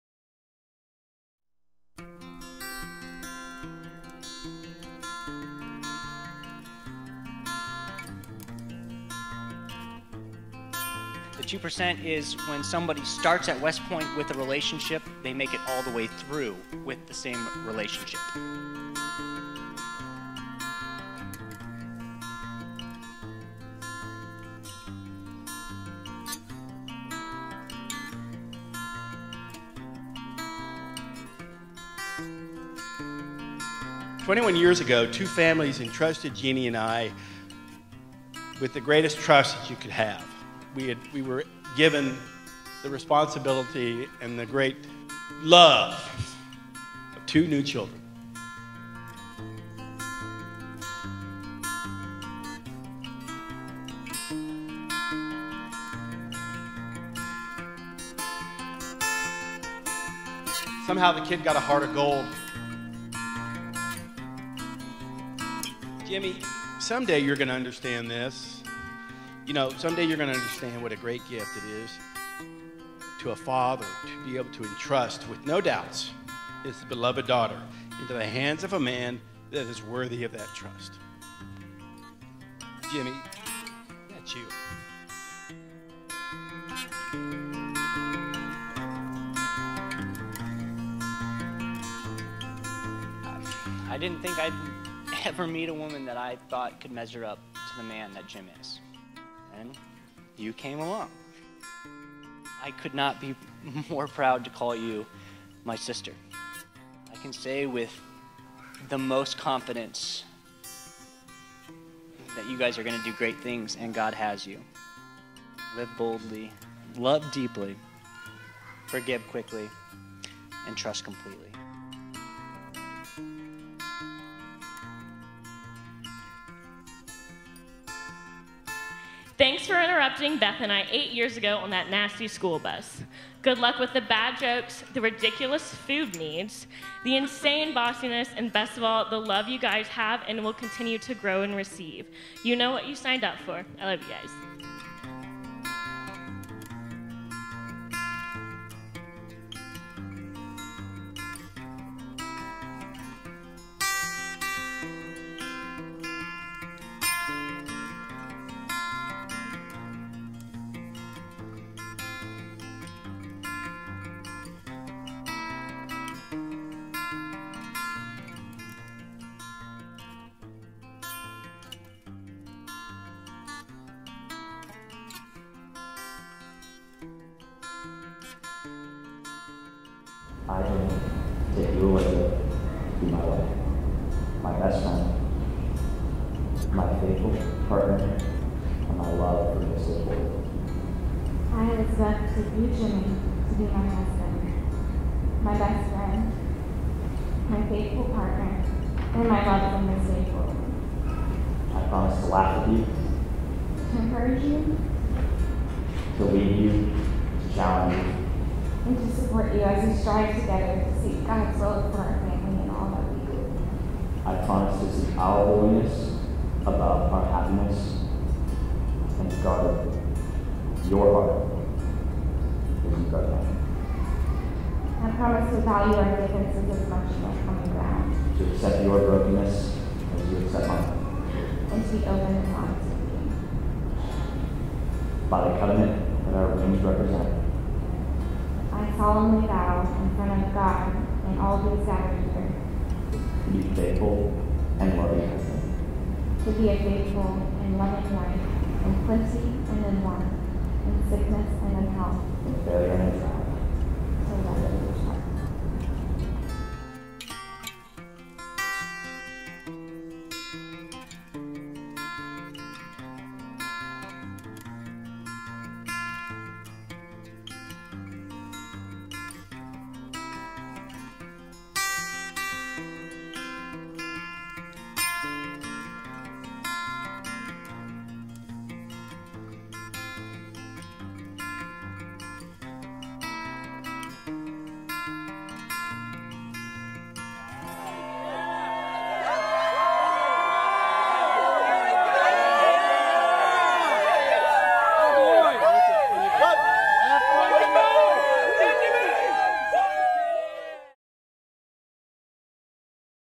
Negley Wedding 6.23.17
We had the privilege of creating this wedding video for some great friends of ours! Congrats Beth and Jimmy!